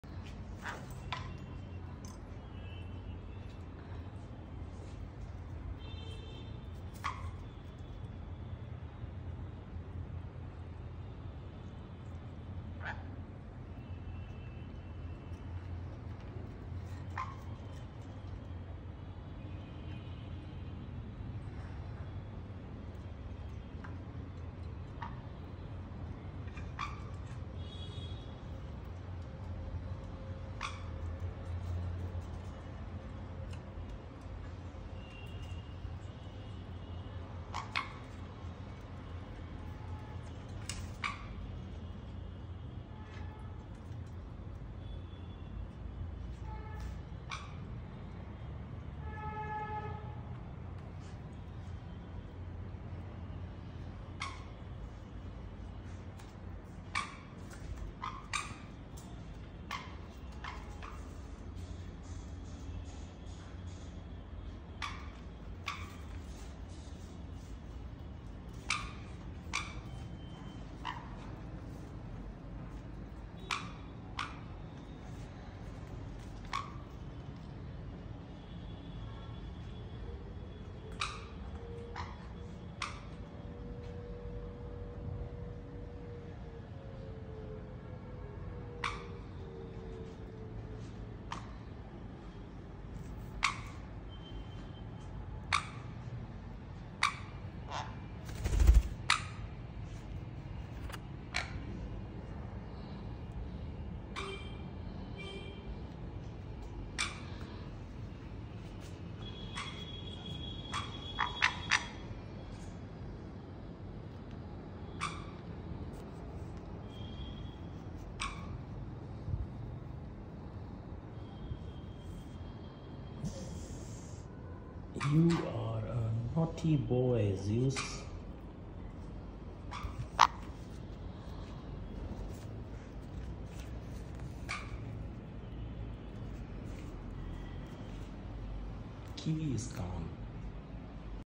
Unclipped Sun Conure pair on play stand
I share my love for parrots and bird care. In this video, you will see my unclipped sun conure pair, Kiwi and Zeus, playing on their play stand.

They are very active and curious birds who love to explore and have fun. They also have a strong bond and enjoy cuddling and grooming each other.

Sun conures are medium-sized parrots that belong to the Aratinga genus of the Psittacidae family

1. They are native to South America and have bright yellow, orange, green, and blue feathers

2. They are very social and vocal birds who need a lot of attention and stimulation.

3. They can also learn to mimic sounds and words with training

Timestamps:
0:00 - Introduction 0:15 - Kiwi and Zeus playing on their play stand 1:00 - Kiwi and Zeus cuddling and grooming 1:45 - Kiwi and Zeus talking and making noises 2:00 - Outro